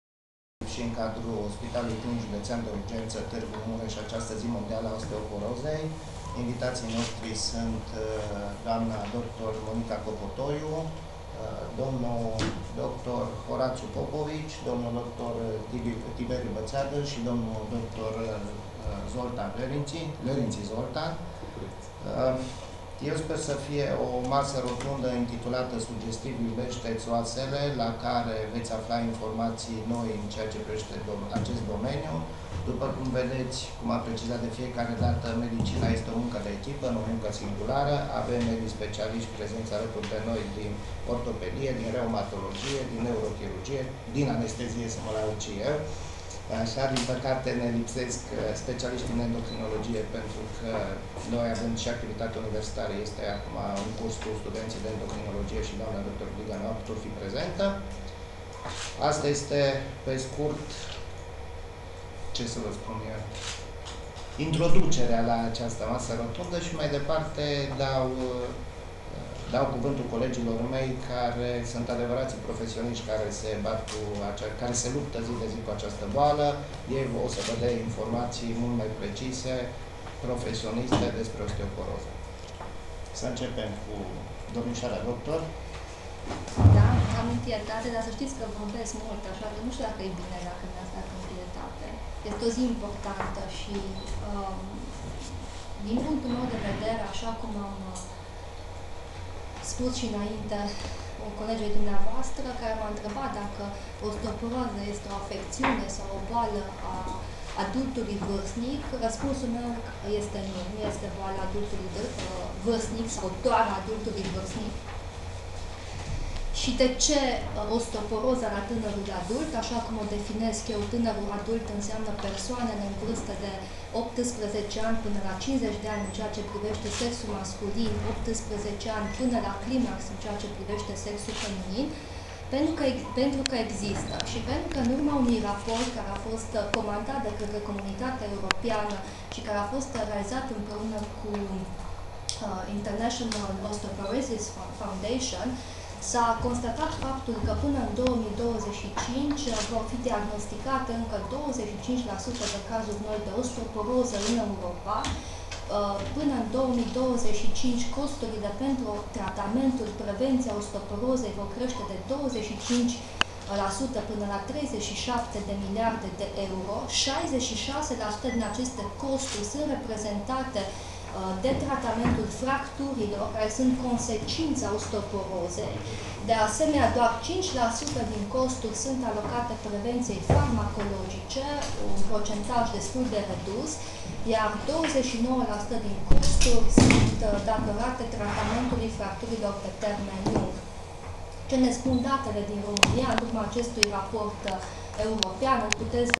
OSTEOPOROZA
"Iubeşte-ţi oasele!" Masă rotundă dedicată Zilei Mondiale a Osteoporozei la Spitalul Clinic Judeţean de Urgenţă din Tîrgu Mureş, 20 oct.2014